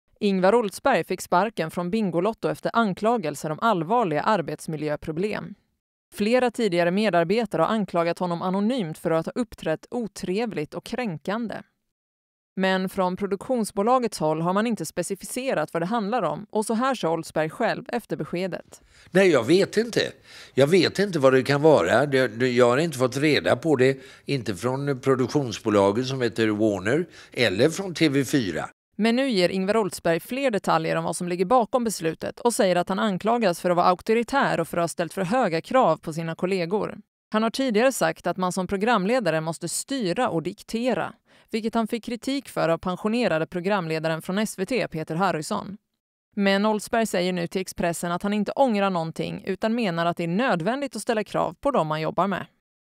Oldsberg sparkades - nu berättar han varför
Ingvar Oldsberg fick sparken från Bingolotto, nu berättar han varför.